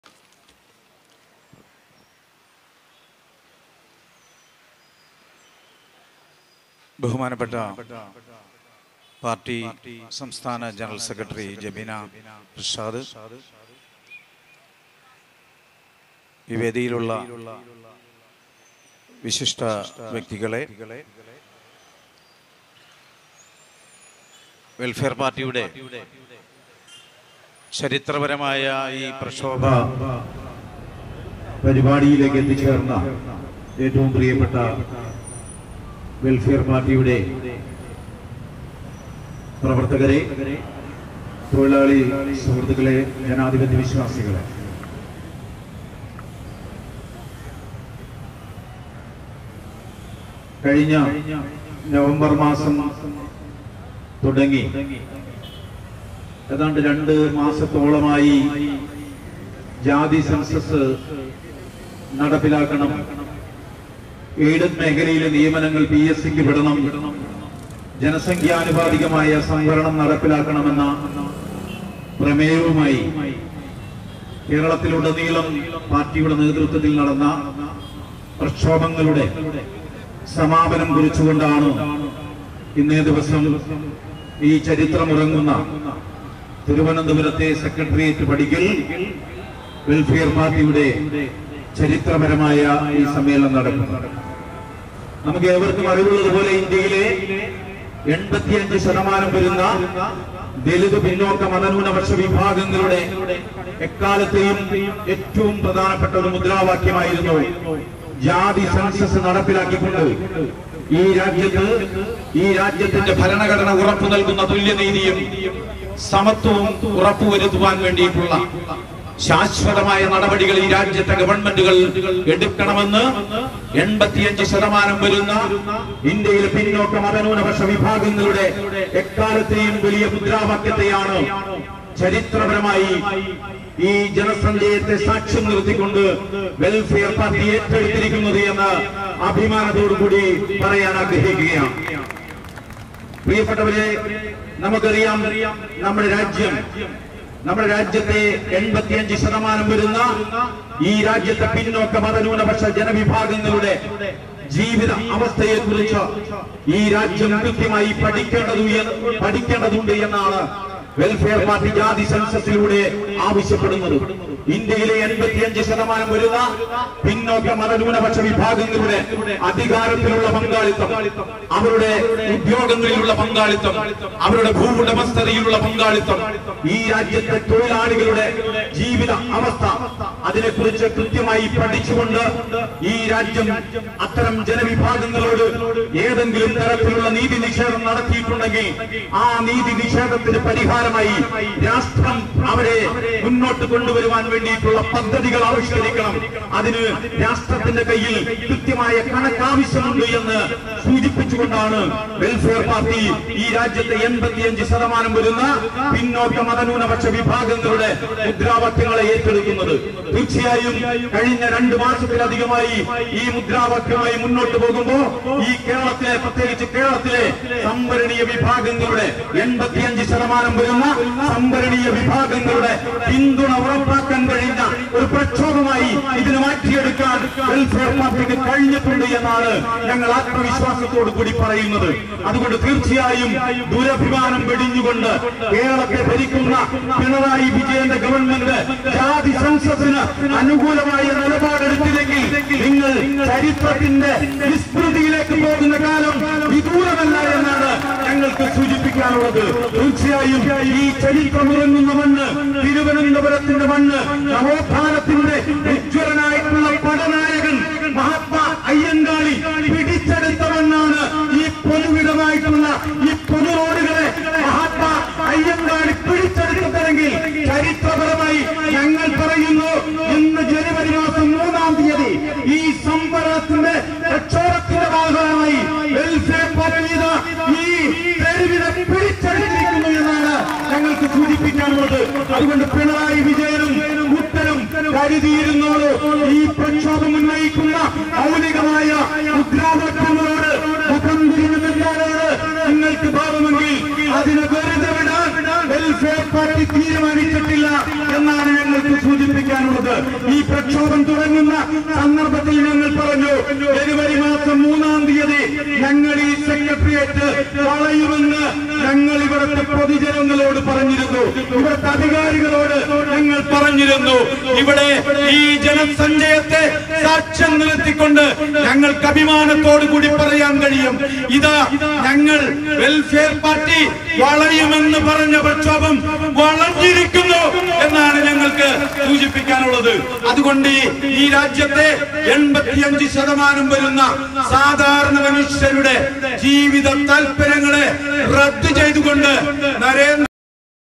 ജ്യോതിവാസ് പറവൂർ | പതിനായിരങ്ങൾ സെക്രട്ടറിയേറ്റ് വളയുന്നു.
▪️ ജാതി സെൻസസ് നടത്തുക.
▪️ എയ്ഡഡ് നിയമനം പി.എസ്.സിക്ക് വിടുക.
▪️ സർക്കാർ സർവ്വീസിൽ ആനുപാതിക   പ്രാതിനിധ്യം നടപ്പാക്കുക.
പതിനായിരങ്ങൾ സെക്രട്ടറിയേറ്റ് വളയുന്നു.
2024 ജനുവരി 3 ബുധൻ
വെൽഫെയർ പാർട്ടി